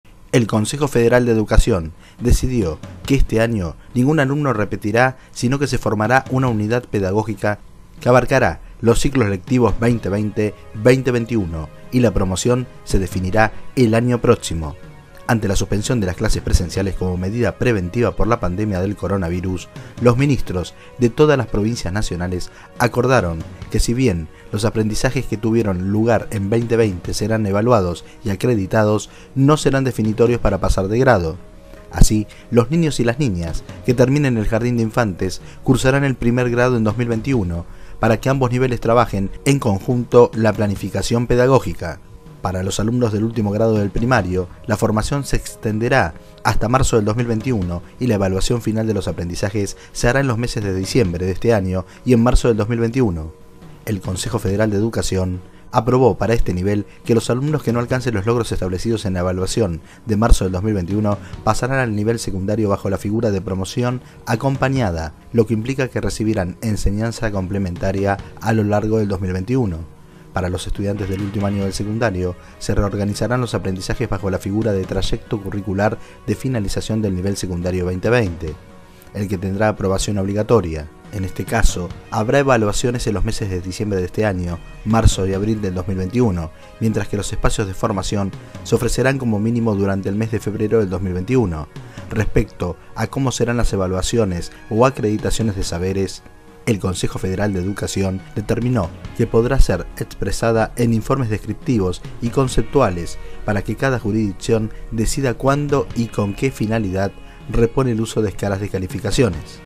NINGUN ALUMNO REPETIRA EL AÑO UNIFICAN EL CICLO LECTIVO 2020 CON EL 2021
EDUCACION